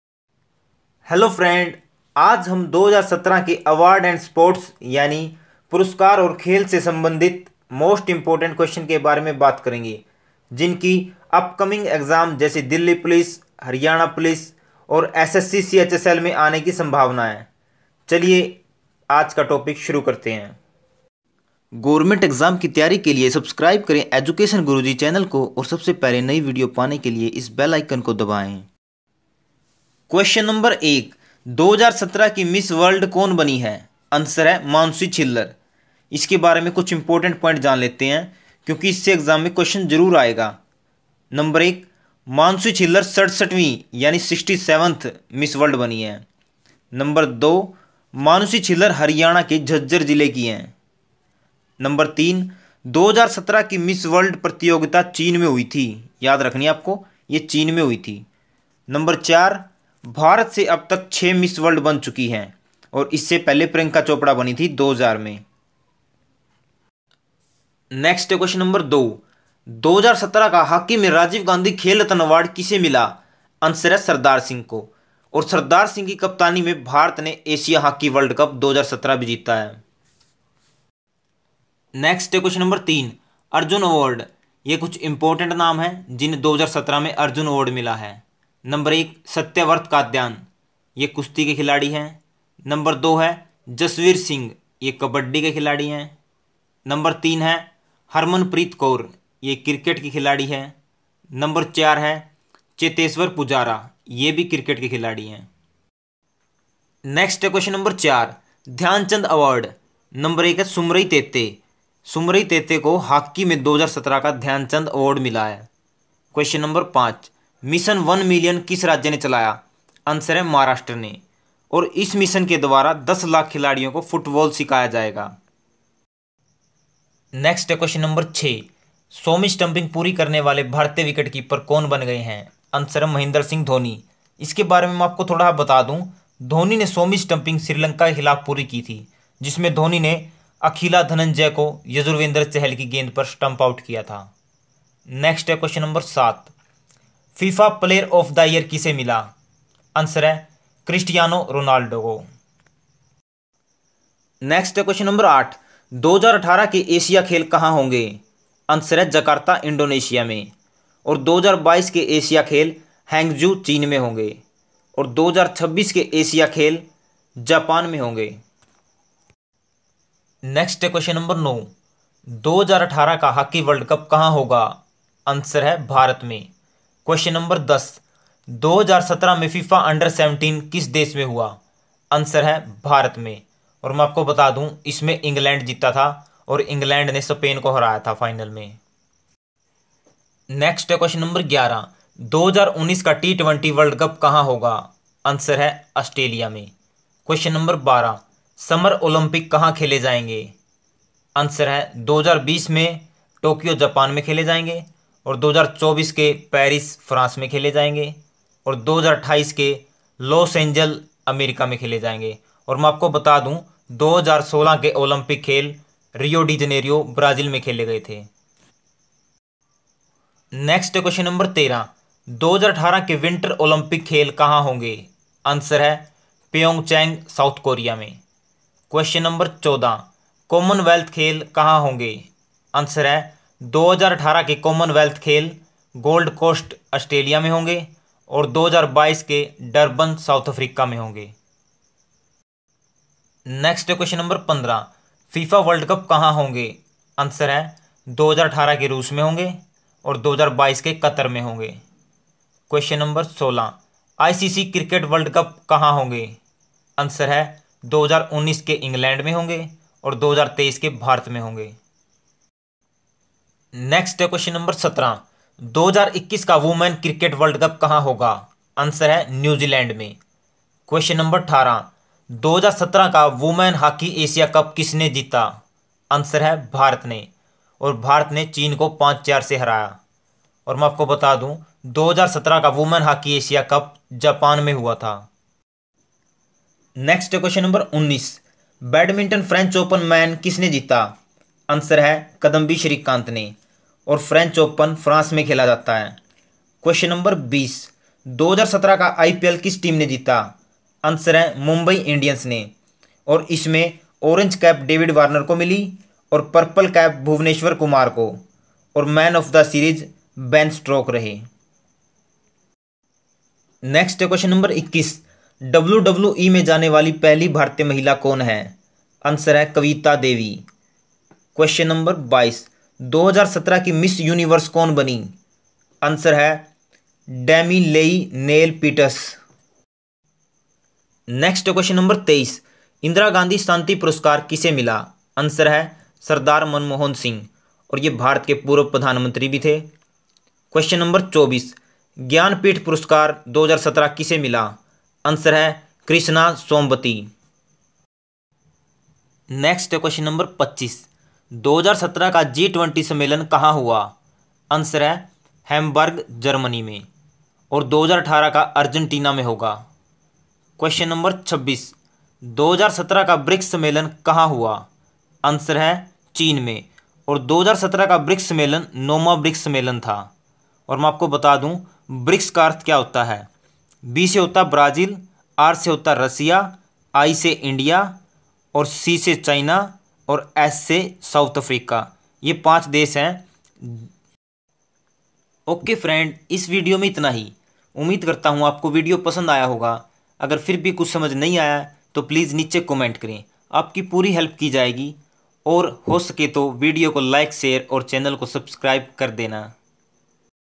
Current affair in hindi for delhi police Awards and sports 2017 खेल और पुरस्कार GK 2017
Namskar dosto, यह video special 2017 के important sports aur awards के current affair का है इस वीडियो के questions की upcoming exam जैसे ssc chsl delhi police haryana police में आने  की पूरी संभावना है

उम्मीद करता हु आपको वीडियो पसंद आएगा
Thanks & Love